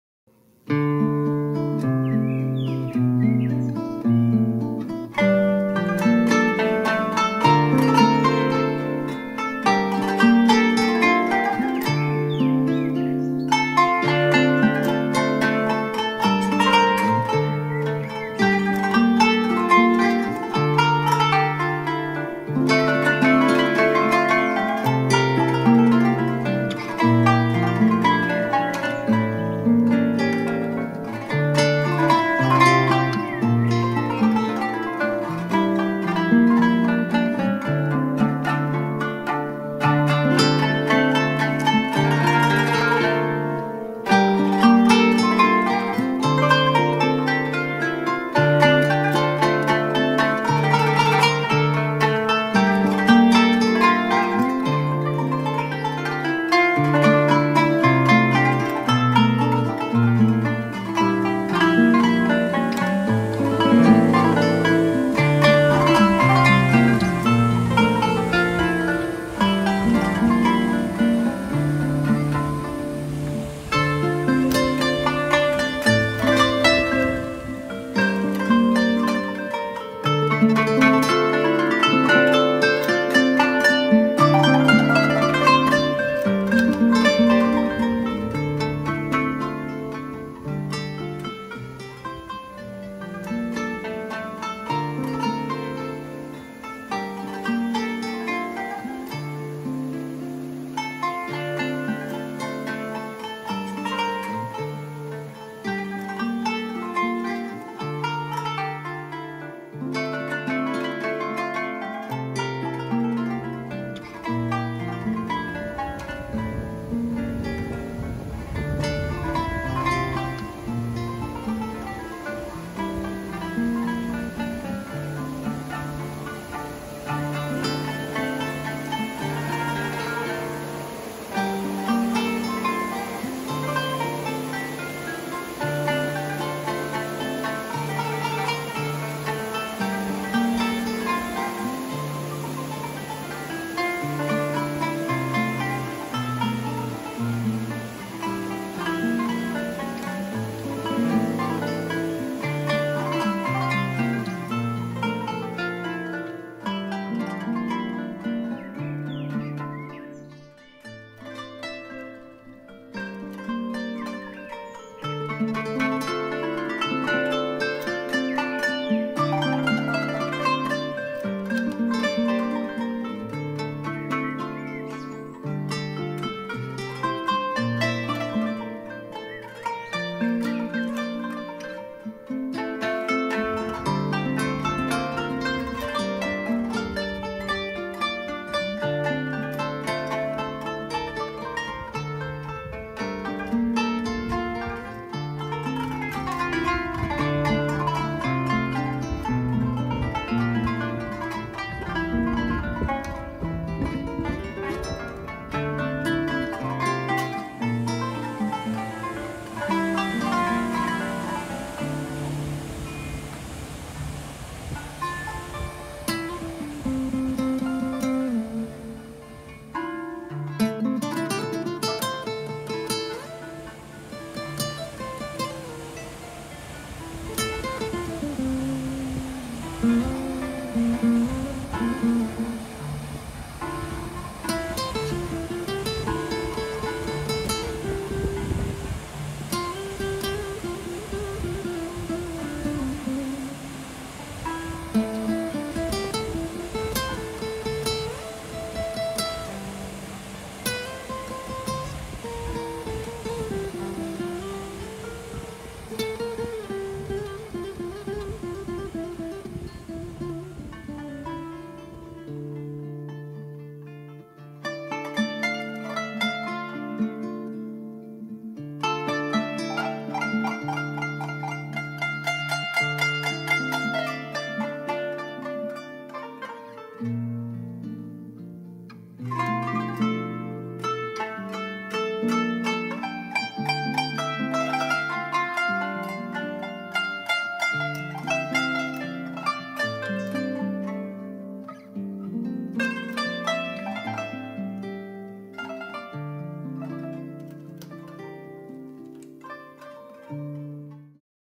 Duden Waterfalls 2024 (Düden şelalesi) - Antalya (Turkey) - HD 1080 (Анталья , أنطاليا)
1:00 Waterfall (Day time)
1:30 Bridge
2:00 River Side cafes
3:00 Park at Sunrise
3:30 Waterfall at Sunrise

This is a tour of Duden Park and Duden Waterfall in Antalya (Turkey).